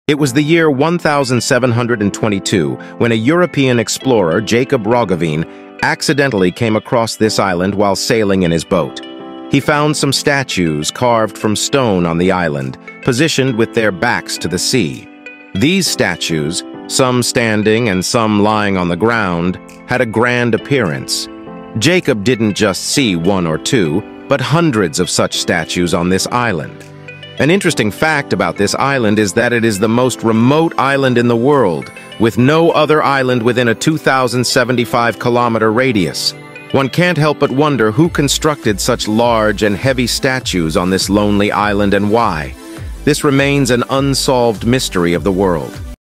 Unsolved mystery of this island#shorts
Discovering the mystery of Easter Island with its impressive stone statues, accidentally stumbled upon by explorer Jacob Roggeveen in Who would have thought this remote island held such ancient treasures?